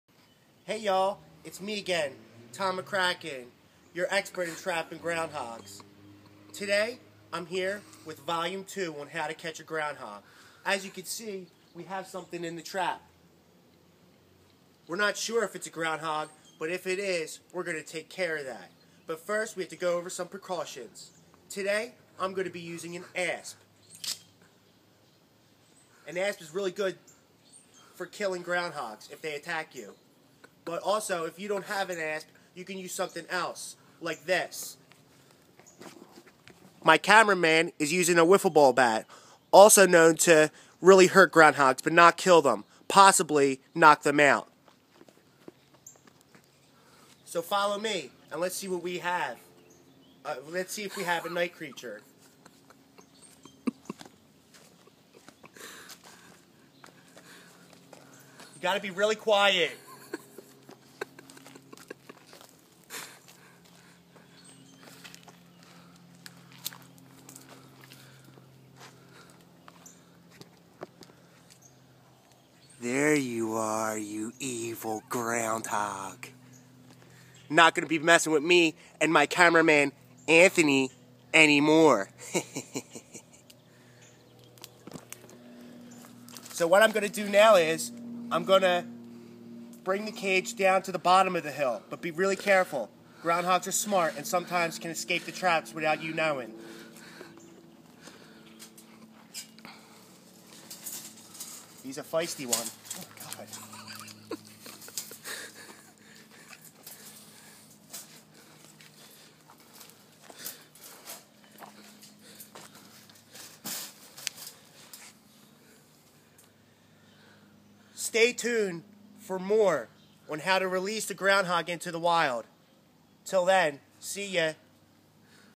To Catch a Groundhog Vol. 2
Now that we caught the groundhog here's an in depth tutorial on how to handle the wild beast!